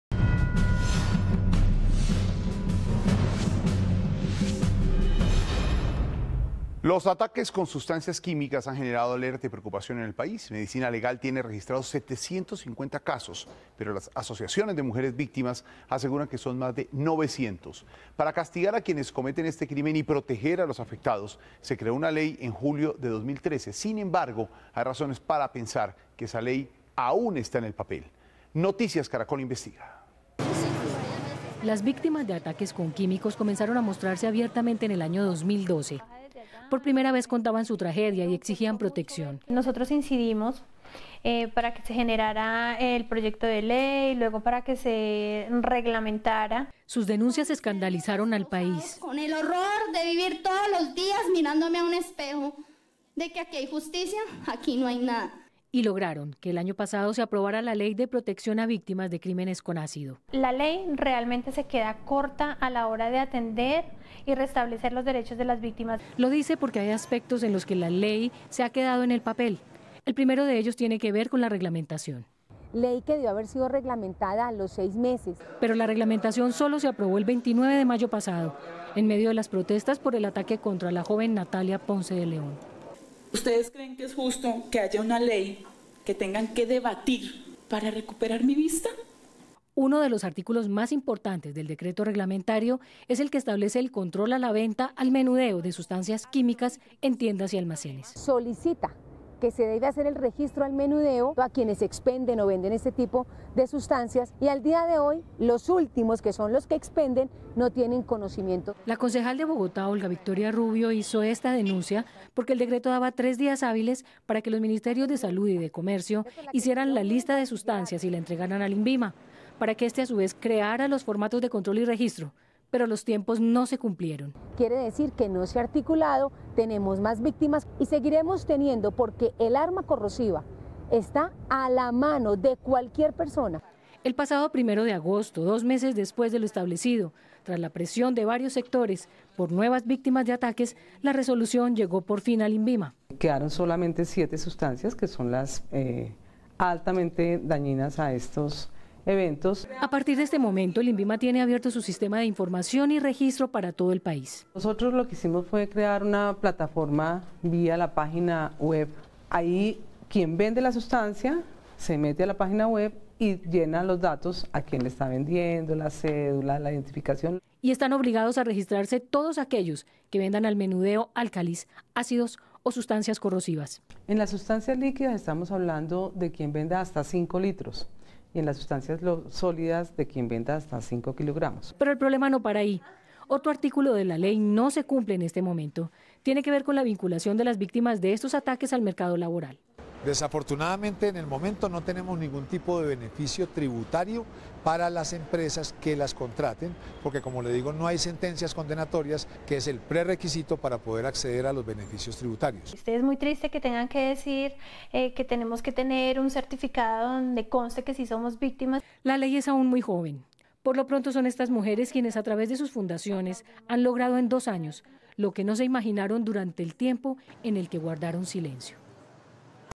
¿Se está cumpliendo ley para proteger a víctimas de ataques de ácido?
Emitido el 07 de Agosto de 2014.